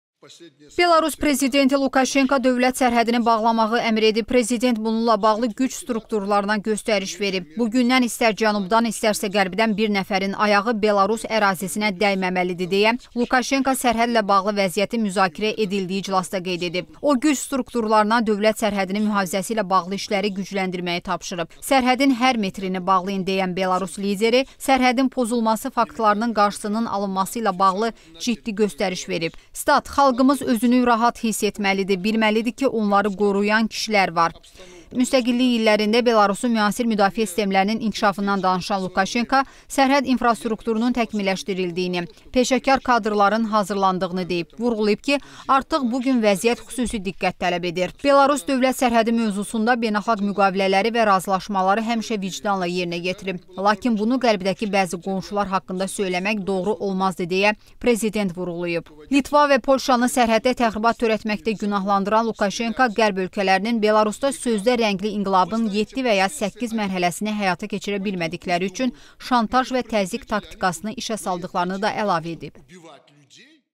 Belarus prezidenti qərb qonşularını ittiham etdi - Baku TV
Belarus Prezidenti Aleksandr Lukaşenko dövlət sərhədini bağlamağı əmr edib. Prezident bununla bağlı güc strukturlarına göstəriş verib.

“Bu gündən istər cənubdan, istərsə Qərbdən bir nəfərin ayağı Belarus ərazisinə dəyməməlidir”, - deyə Lukaşenko sərhədlə bağlı vəziyyətin müzakirə edildiyi iclasda qeyd edib.

O, güc strukturlarına dövlət sərhədinin mühafizəsi ilə bağlı işləri gücləndirməyi tapşırıb. “Sərhədin hər metrini bağlayın” deyən Belarus lideri sərhədin pozulması faktlarının qarşısının alınması ilə bağlı ciddi göstəriş verib.

Müstəqillik illərində Belarusun müasir müdafiə sistemlərinin inkişafından danışan Lukaşenko sərhəd infrastrukturunun təkmilləşdirildiyini, peşəkar kadrların hazırlandığını deyib. Vurğulayıb ki, artıq bu gün vəziyyət xüsusi diqqət tələb edir.

“Belarus dövlət sərhədi mövzusunda beynəlxalq müqavilələri və razılaşmaları həmişə vicdanla yerinə yetirib. Lakin bunu Qərbdəki bəzi qonşular haqqında söyləmək doğru olmazdı”, - deyə Prezident vurğulayıb.

Litva və Polşanı sərhəddə təxribat törətməkdə günahlandıran Lukaşenko Qərb ölkələrinin Belarusda sözdə rəngli inqilabın 7 və ya 8 mərhələsini həyata keçirə bilmədikləri üçün şantaj və təzyiq taktikasını işə saldıqlarını da əlavə edib.